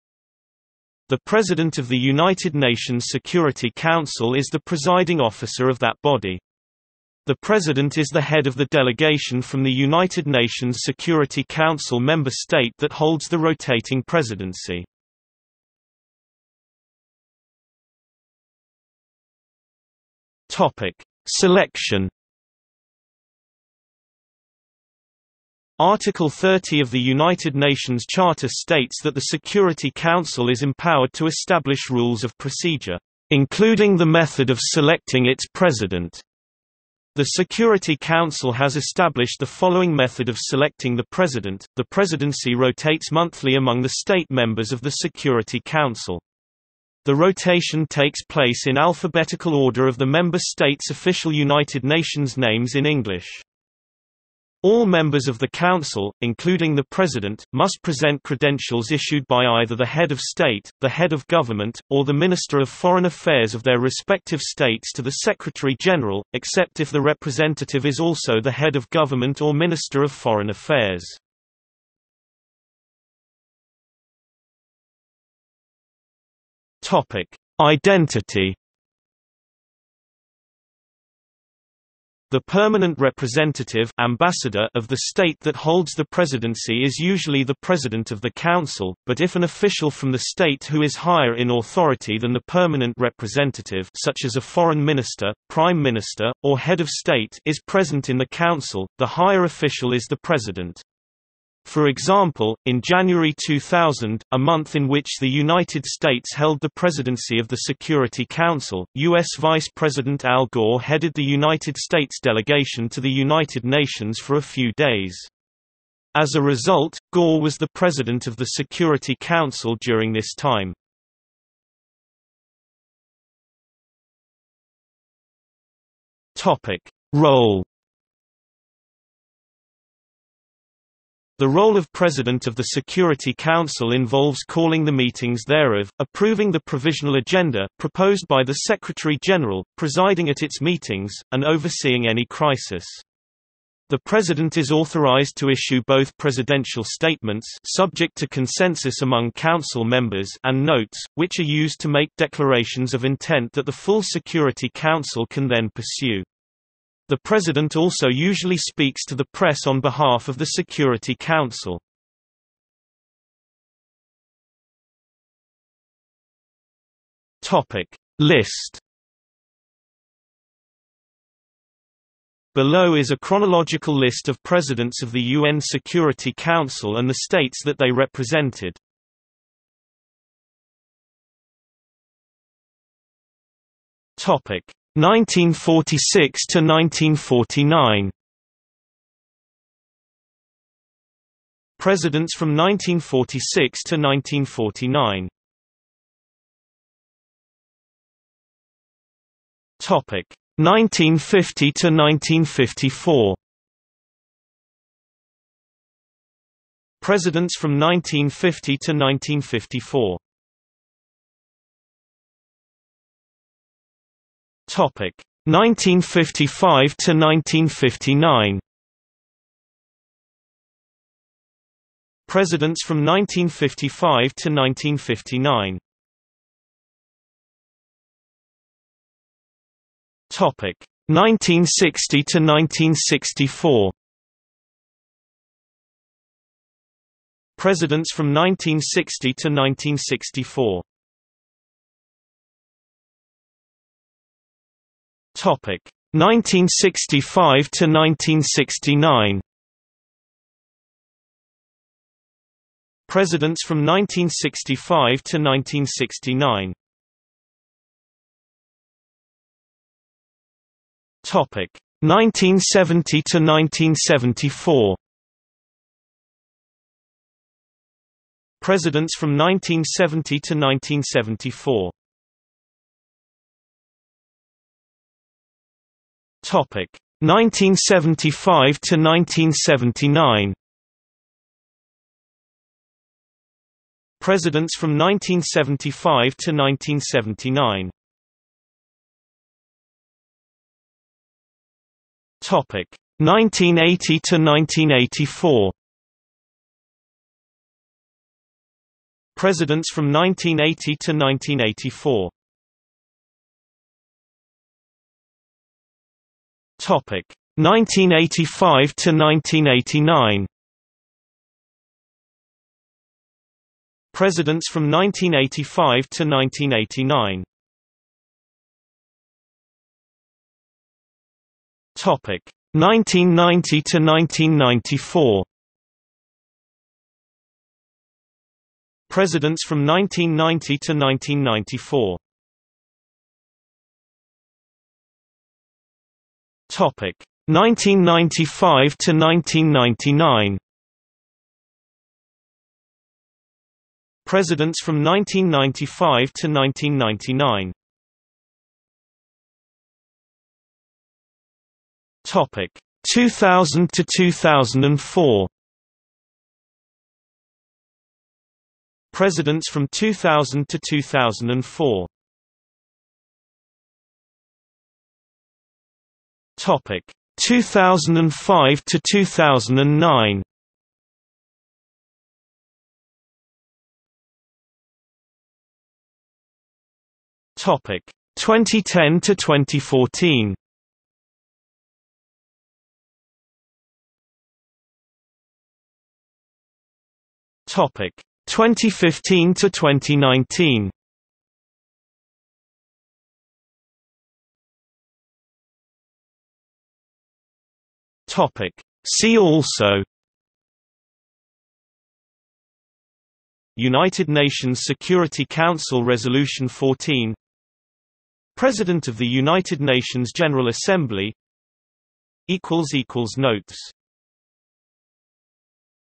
President of the United Nations Security Council | Wikipedia audio article
00:00:16 1 Selection
00:01:13 2 Identity
00:02:02 3 Role
00:02:43 4 List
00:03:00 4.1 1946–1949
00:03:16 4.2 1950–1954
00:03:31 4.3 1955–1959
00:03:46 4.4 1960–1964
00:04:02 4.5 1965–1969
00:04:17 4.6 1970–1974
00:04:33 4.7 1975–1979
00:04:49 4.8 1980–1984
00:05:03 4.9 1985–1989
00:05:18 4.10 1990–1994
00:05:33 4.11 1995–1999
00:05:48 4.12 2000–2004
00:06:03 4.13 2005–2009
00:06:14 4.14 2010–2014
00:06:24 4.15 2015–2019
00:06:34 5 See also

- Socrates

SUMMARY
The President of the United Nations Security Council is the presiding officer of that body. The President is the head of the delegation from the United Nations Security Council member state that holds the rotating presidency.